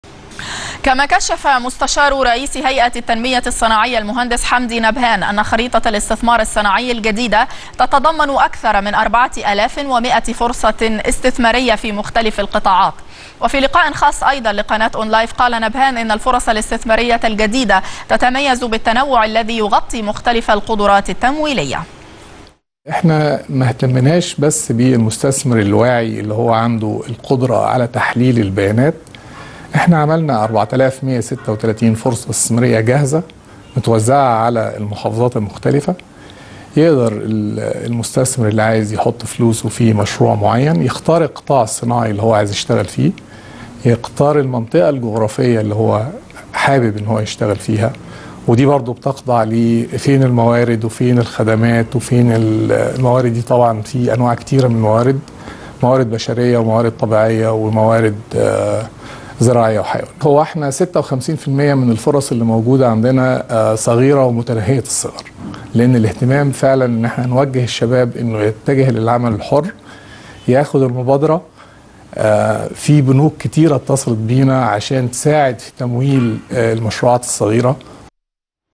م. حمدي نبهان: خريطة الاستثمار الصناعي الجديدة تضم 4136 فرصة استثمارية
الموقع الرسمي للقناة  ..